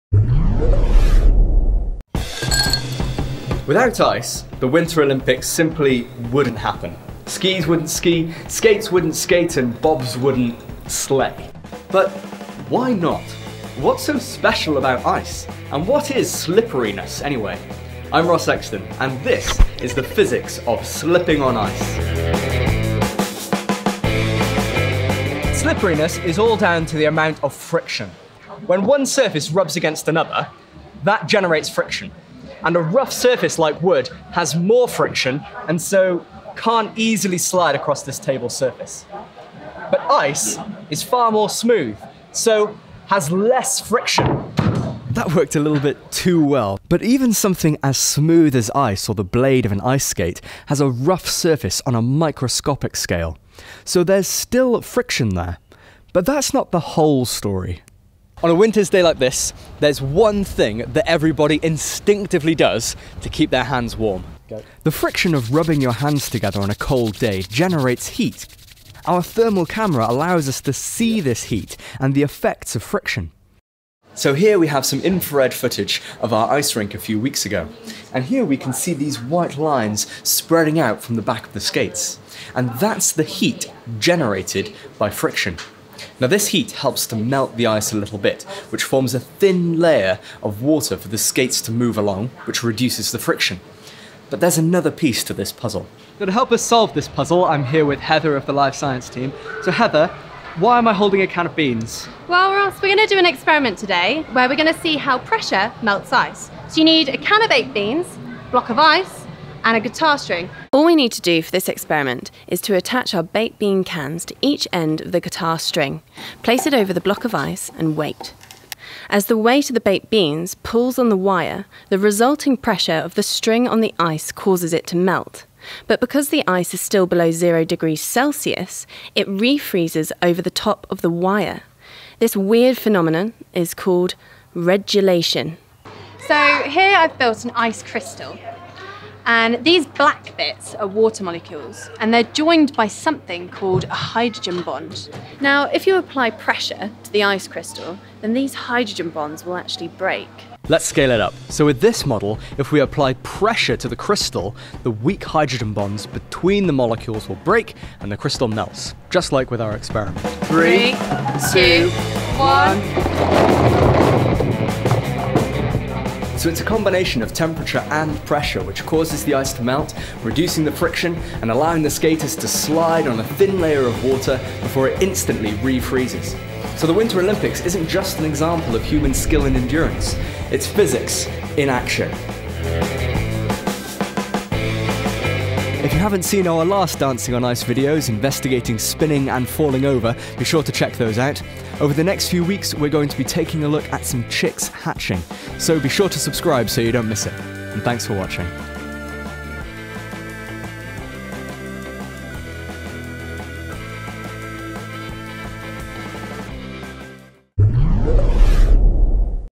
Dancing on Ice: Why is Ice Slippery? | 2014 Winter Olympics | We The Curious
Without ice the Sochi Winter Olympics wouldn't happen. But why not? How does an ice skate work? Join Ross Exton as he investigates the physics of ice-skating, and the secret of regelation.

This video was presented by: Ross Exton
Produced by: Ross Exton & Seamus Foley
Special thanks to: Suzanne Shaw, Matt Evers & ITV's Dancing on Ice, 2014
Live Science Team: Heather Nichol
Skaters: Jodie Elliott & Jazz Davies
Thanks to: Jimmy, volunteer